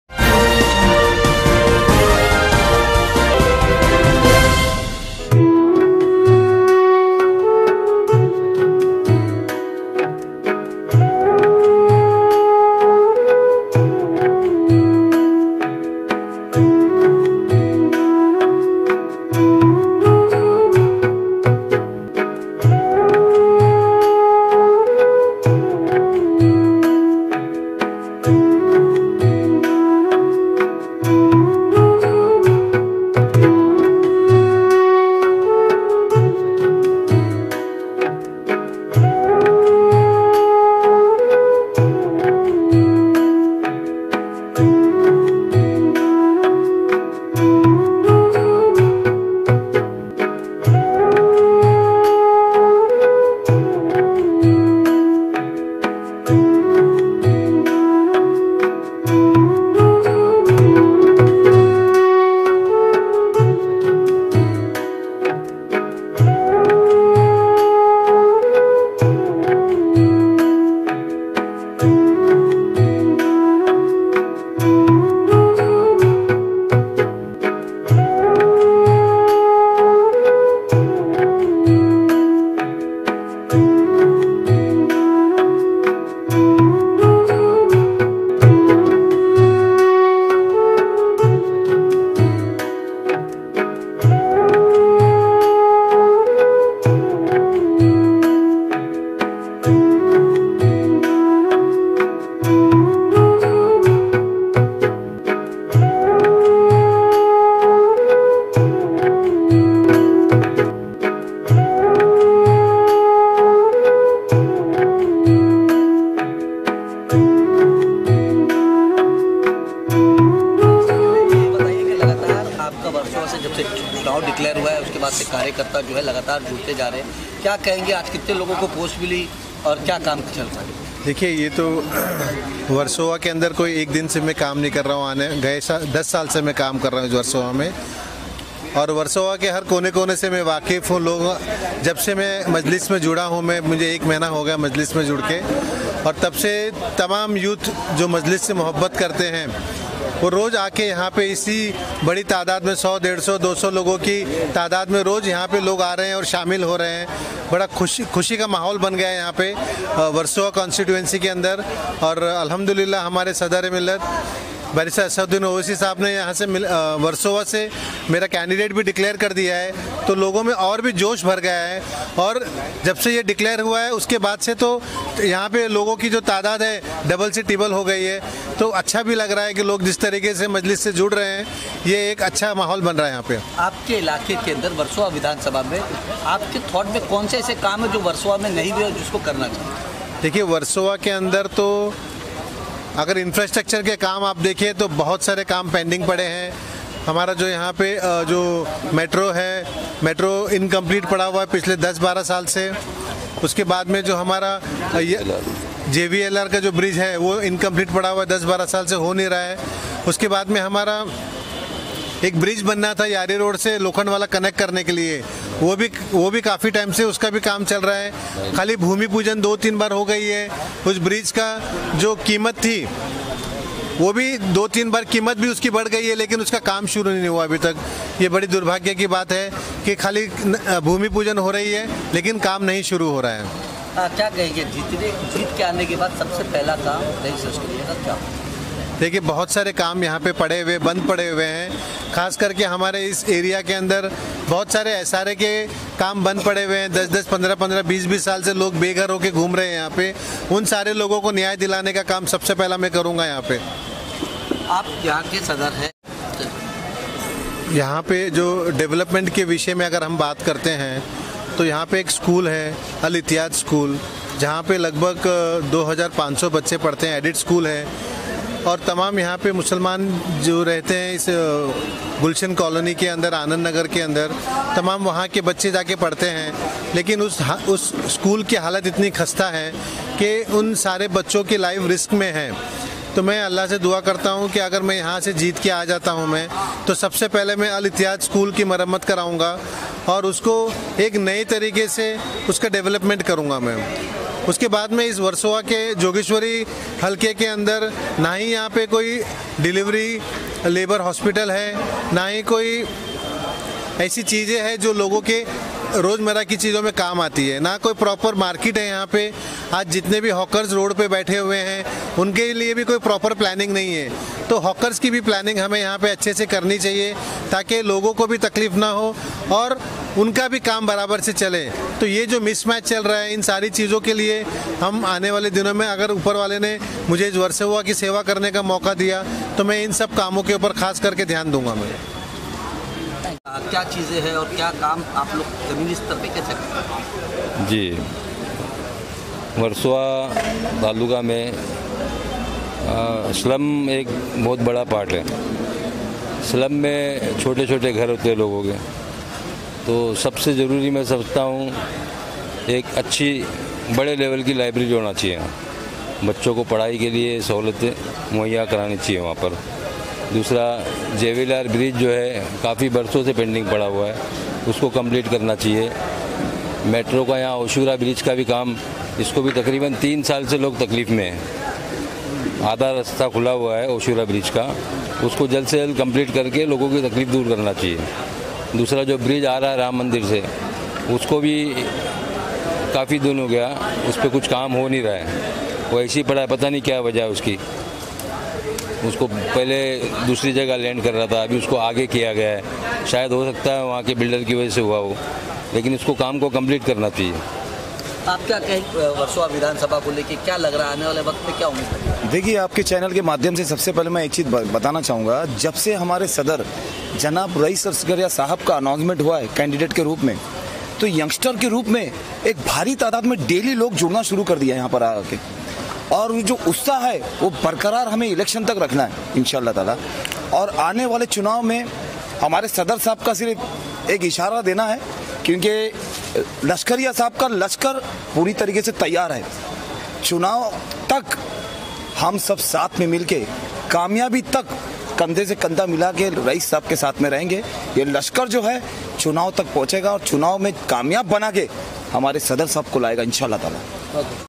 Raees Lashkaria ने नवजवानों को Guid किया एवम AIMIM को मुंबई में मजबूत करने पर की चर्चा
Raees Lashkaria ने  नवजवानों को Guid किया एवम AIMIM को मुंबई में मजबूत करने पर की चर्चा @Khabar24